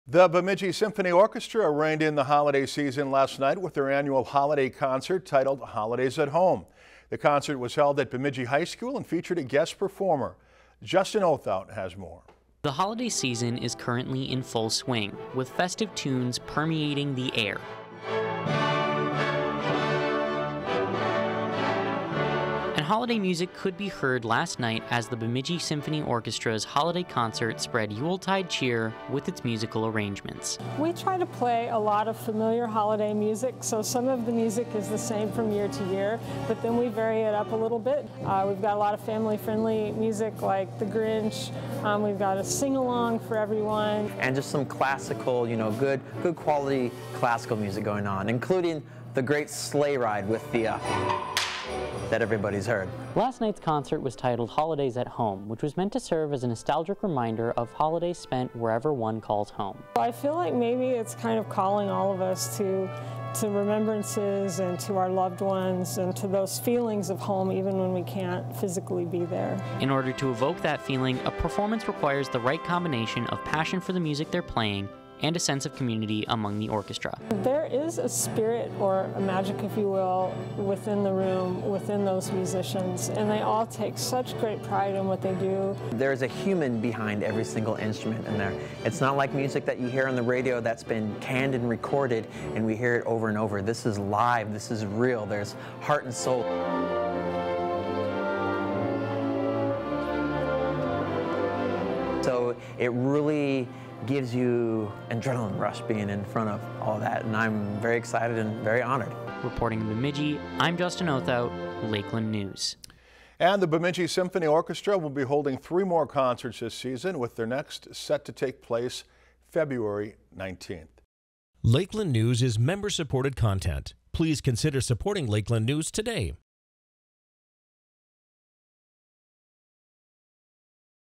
Bemidji Symphony Orchestra Rings in Holiday Season with 'Holidays at Home' Concert
Originally aired December 7, 2022 on Lakeland News